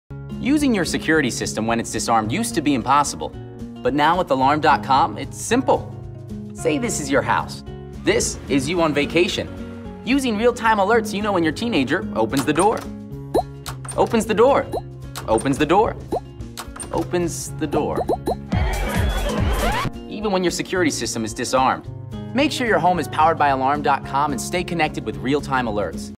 So Smart, It's Simple - Real Time Alerts
No matter where you are suretyCAM, powered by Alarm.com, connects you to what's happening at home. Real-time notifications on your smart phone keep you aware of the important things, even when your security system is disarmed.